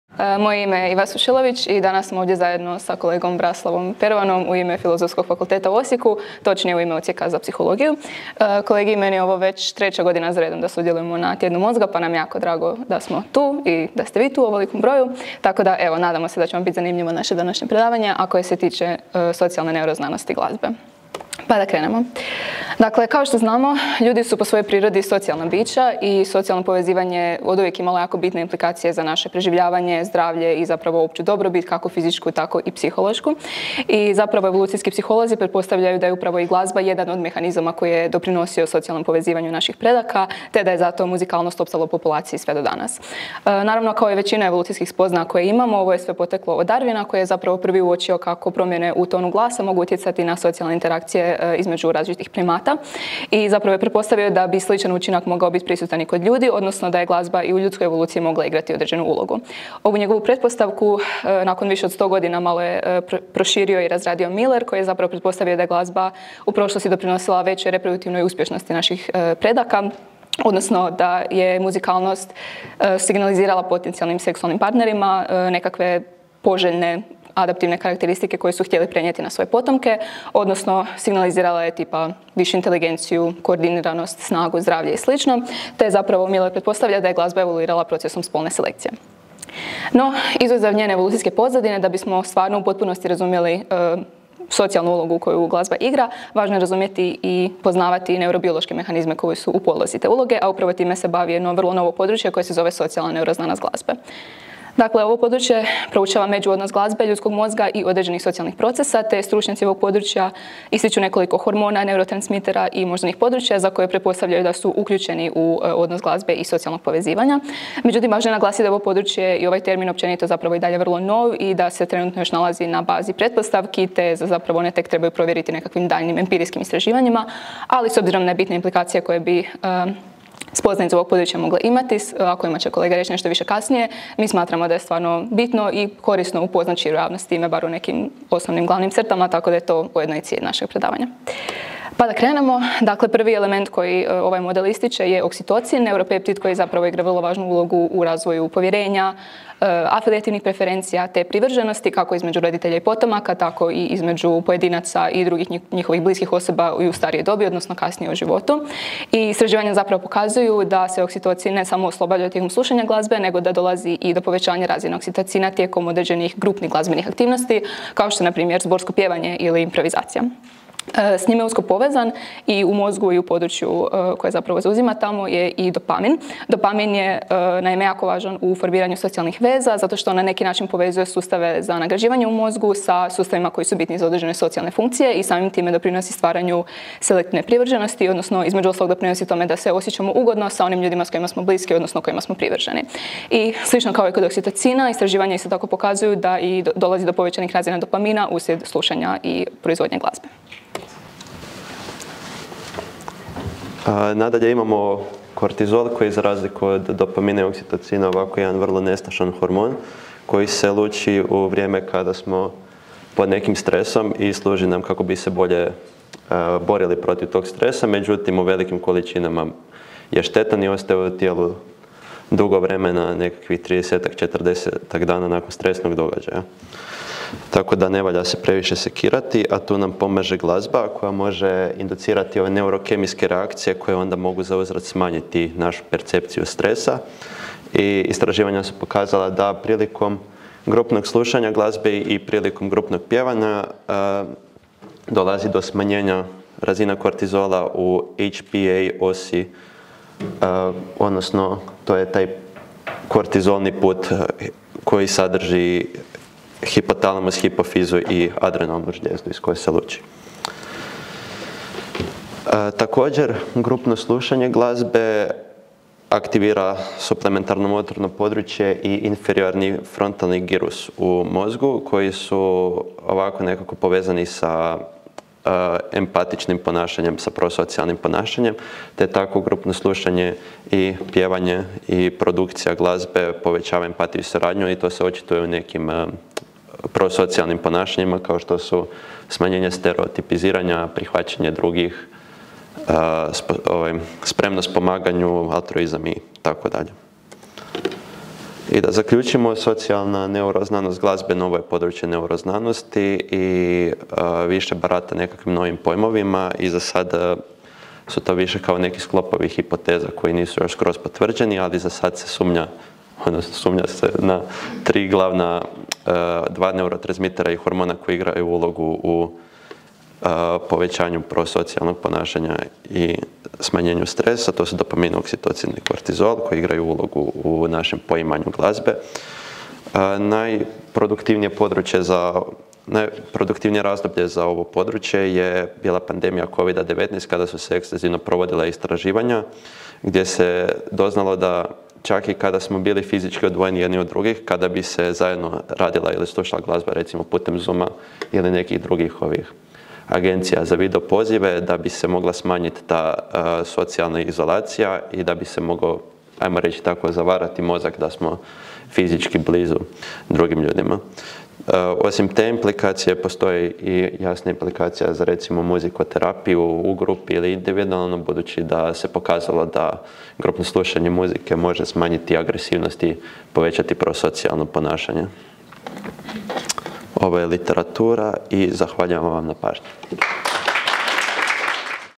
Sada pratim ritam tvoj: socijalna neuroznanostglazbe
Sada pratim ritam tvoj: socijalna neuroznanost
glazbe

Iva Sušilović, Braslav Pervan

Iako je već dugo poznato da glazba za ljude ima određenu
adaptivnu funkciju te da na mnoge načine doprinosi fizičkom i
mentalnom zdravlju, malo je toga poznato o socijalnoj
neuroznanosti u podlozi glazbe. Stoga je cilj ovog predavanja na
zanimljiv način prenijeti slušateljima najvažnije spoznaje ovog
područja istraživanja. Točnije, predavanje će se osvrnuti na
neurobiološke mehanizme u podlozi stvaranja glazbe, kao i
njihovu ulogu u socijalnom povezivanju.